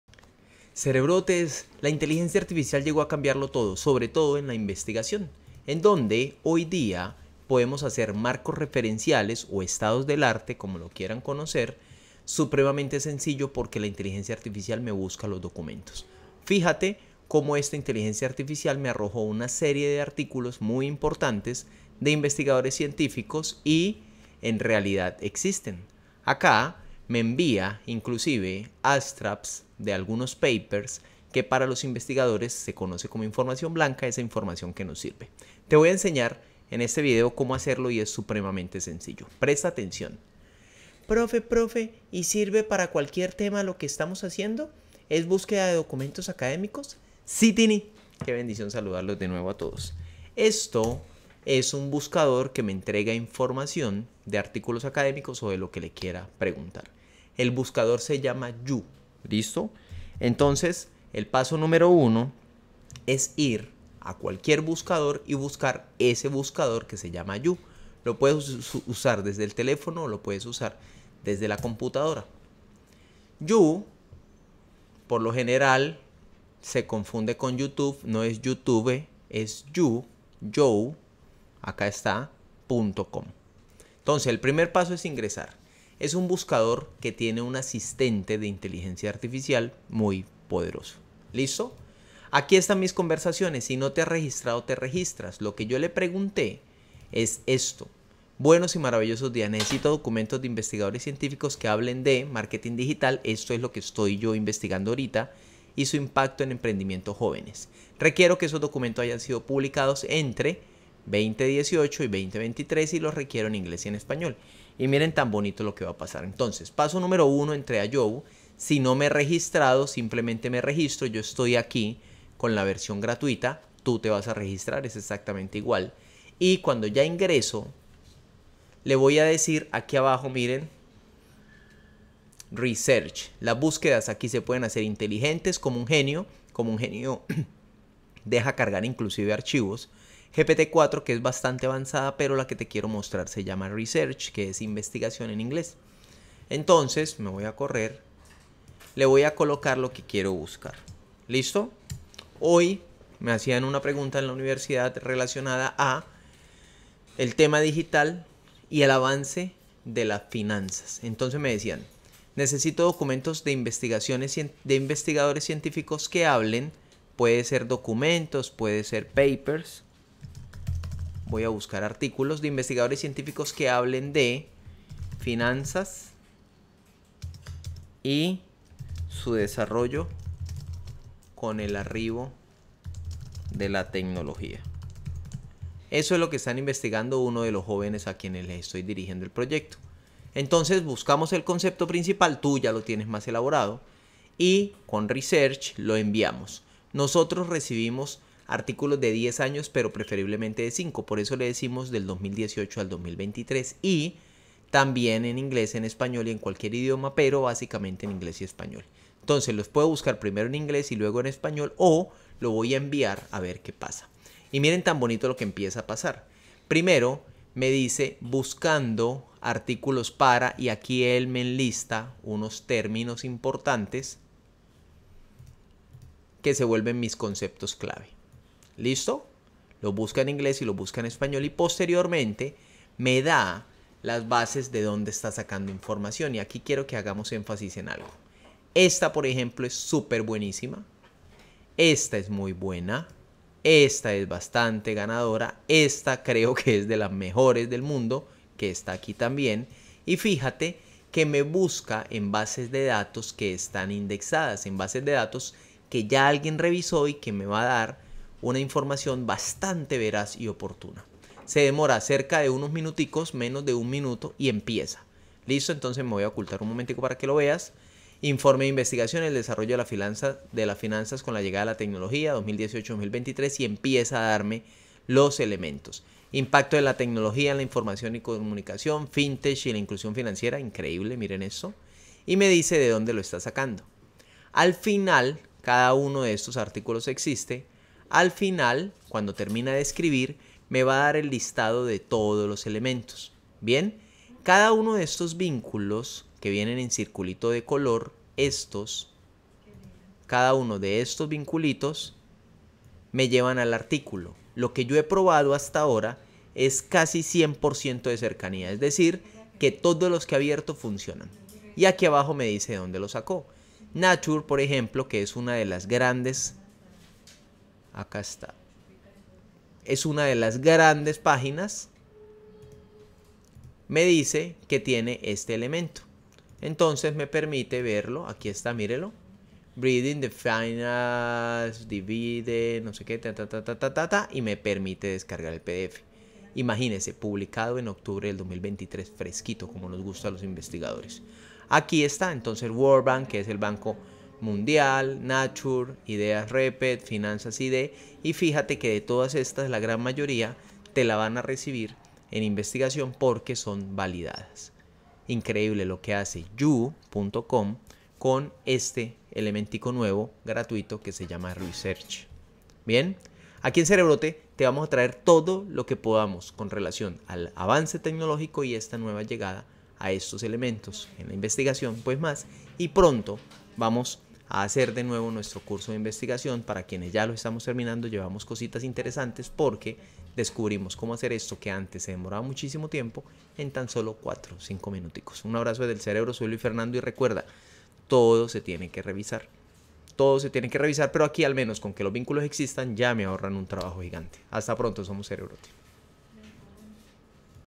Marcos referenciales para investigación en 5 minutos con IA You com
Con la llegada de la inteligenciaartificial las cosas cambiaron bastante, ahora con You.com se puedes conseguir artículos académicos de alta calidad en menos de 5 minutos tal como lo ves en el video que el profes Luis Fernando Botero comparte acá en Cerebrote.